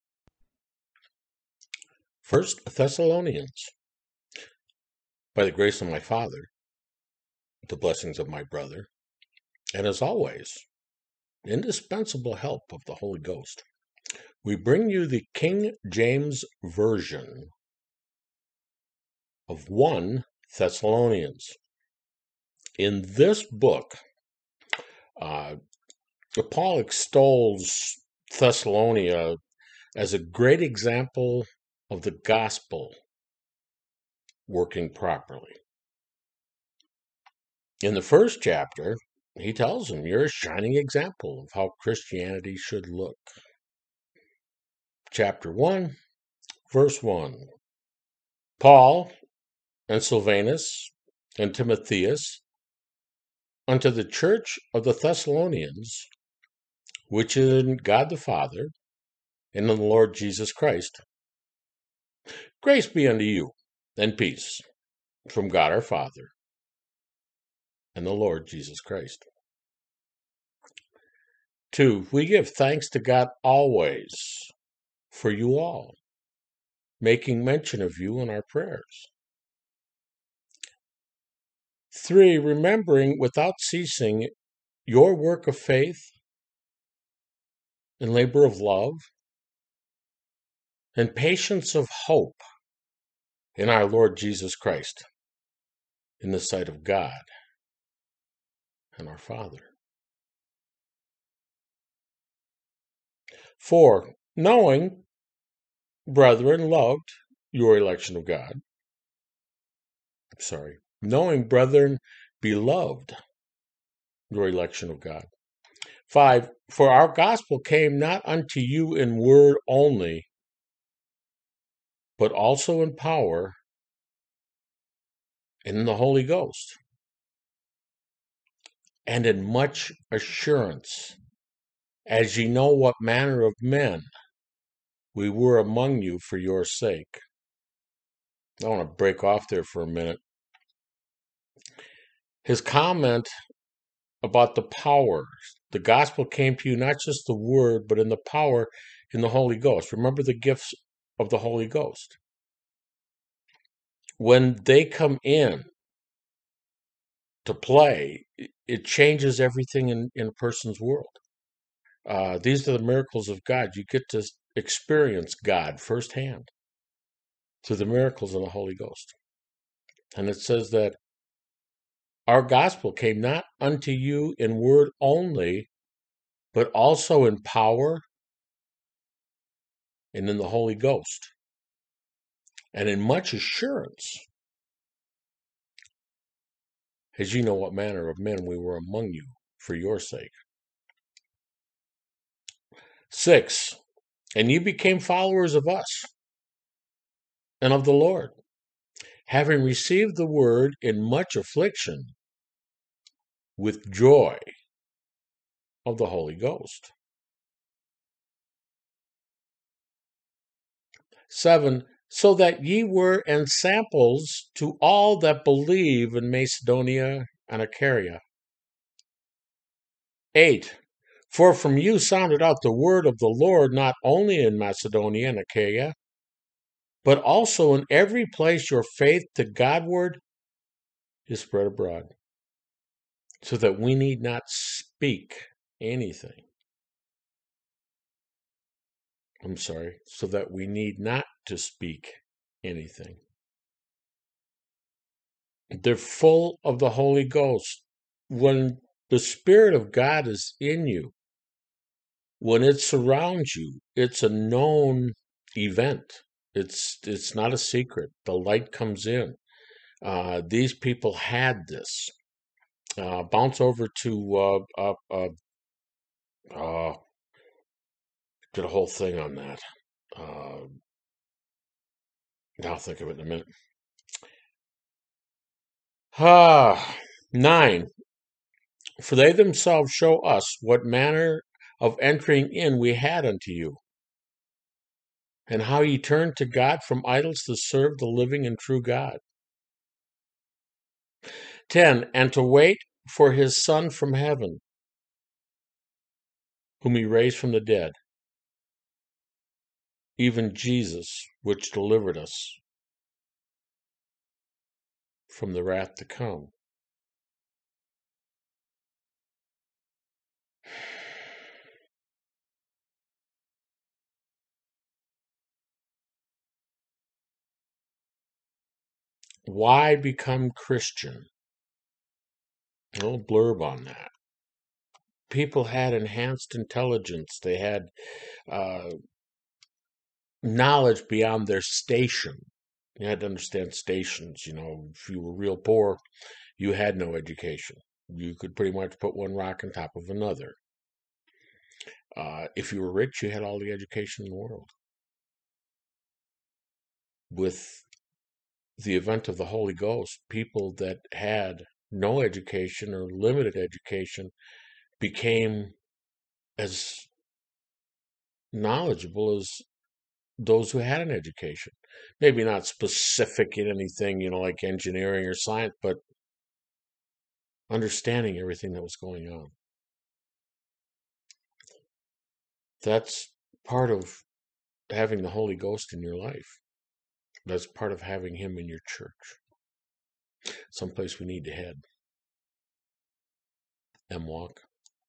1 Thessalonians - Paul extols this as a great example of the gospel working properly
1 Thessalonians 1  -   You are a shining example of how Christianity should look in action

[1] Paul, and Silvanus, and Timotheus, unto the church of the Thessalonians which is in God the Father and in the Lord Jesus Christ: Grace be unto you, and peace, from God our Father, and the Lord Jesus Christ.
[2] We give thanks to God always for you all, making mention of you in our prayers;
[3] Remembering without ceasing your work of faith, and labour of love, and patience of hope in our Lord Jesus Christ, in the sight of God and our Father;
[4] Knowing, brethren beloved, your election of God.
[5] For our gospel came not unto you in word only, but also in power, and in the Holy Ghost, and in much assurance; as ye know what manner of men we were among you for your sake.
[6] And ye became followers of us, and of the Lord, having received the word in much affliction, with joy of the Holy Ghost:
[7] So that ye were ensamples to all that believe in Macedonia and Achaia.
[8] For from you sounded out the word of the Lord not only in Macedonia and Achaia, but also in every place your faith to God-ward is spread abroad; so that we need not to speak any thing.
[9] For they themselves shew of us what manner of entering in we had unto you, and how ye turned to God from idols to serve the living and true God;
[10] And to wait for his Son from heaven, whom he raised from the dead, even Jesus, which delivered us from the wrath to come.